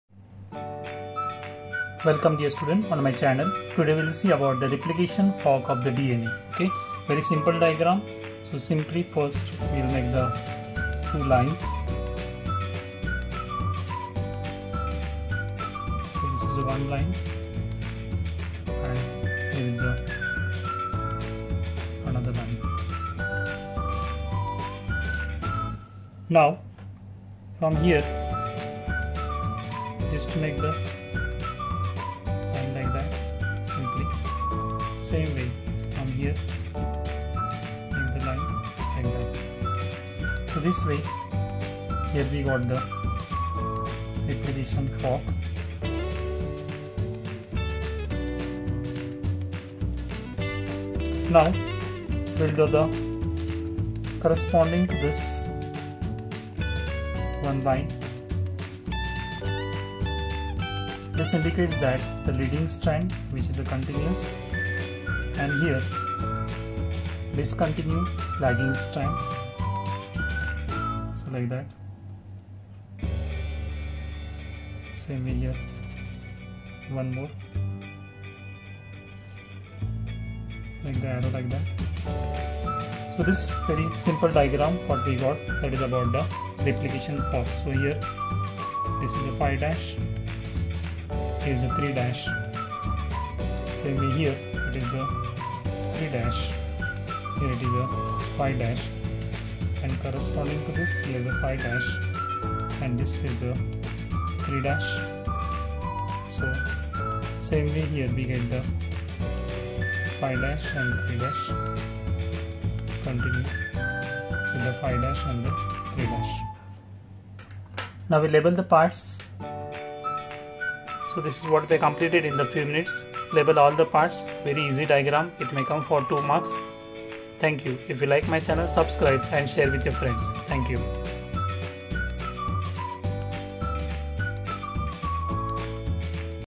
How to draw DNA Replication fork Class-12 (Imp. Diagram for 2021) Easy Steps By Prof. Prakash Surve
Easy steps to draw Brain Diagram

link for live demonstration of biology videos

link for Biology Practical videos

link for How to draw diagrams easily